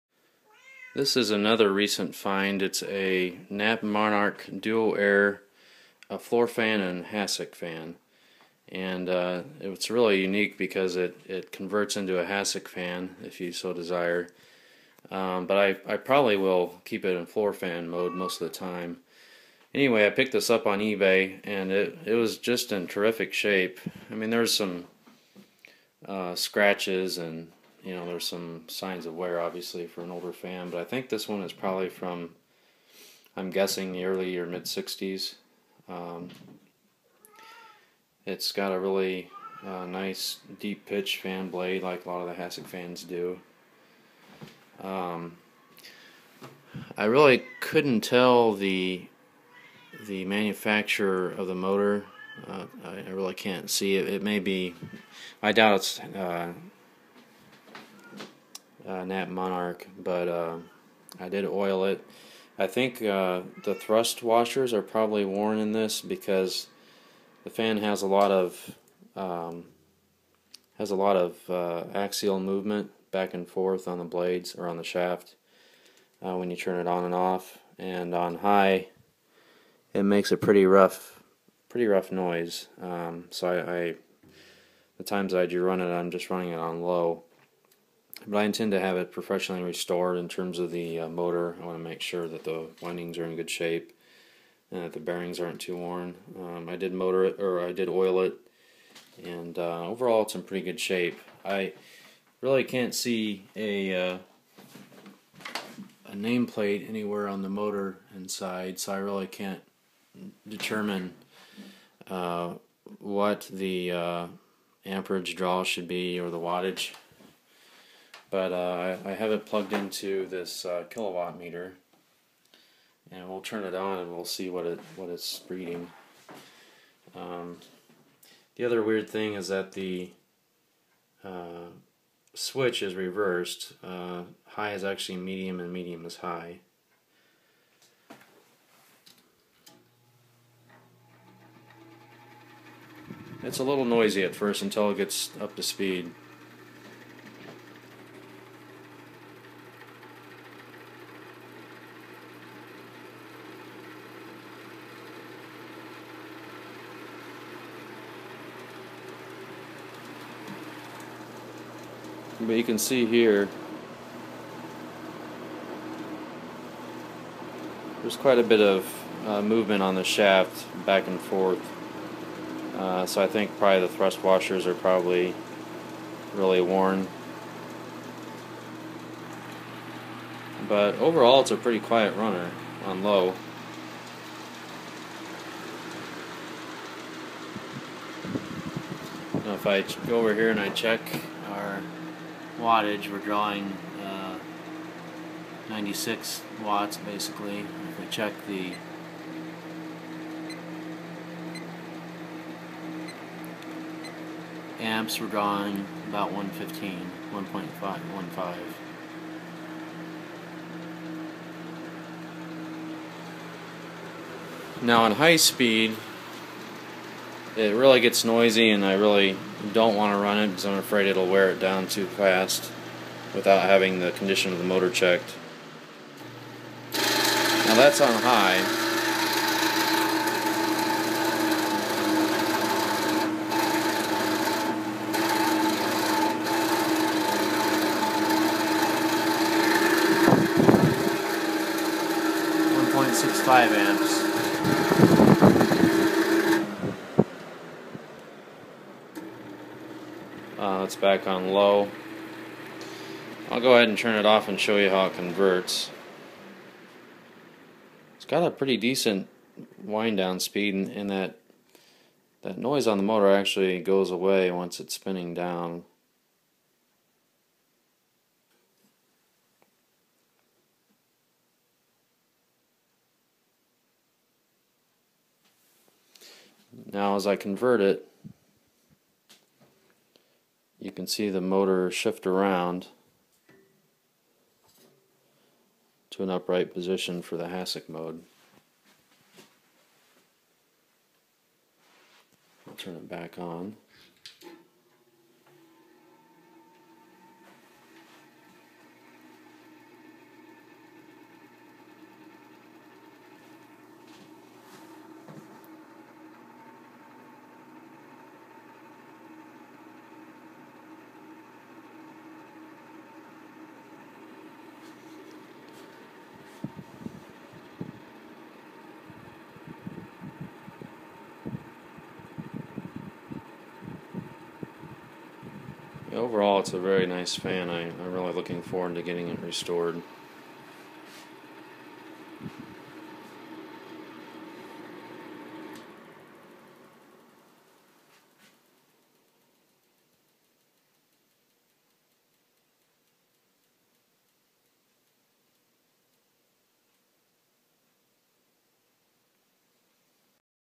Knapp Monarch Duo Aire Hassock/Floor Fan
I recently acquired this Knapp Monarch Duo-Aire fan  that is in overall, really good shape. However, I believe the thrust washer(s) is/are worn as the shaft has a lot of axial movement. I plan on having the windings checked and re-dipped as well as the finish restored. I plugged the fan into a Kill-O-Watt meter to check the current draw but unfortunately the motor has no spec tag.